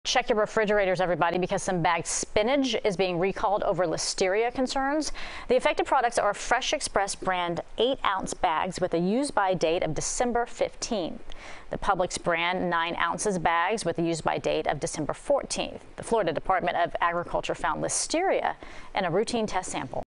Bagged spinach recalled over listeria concerns
Fresh Express brand 8 ounce bags with a use by date of December 15th and Publix 9 ounce bags with a use by date of December 14th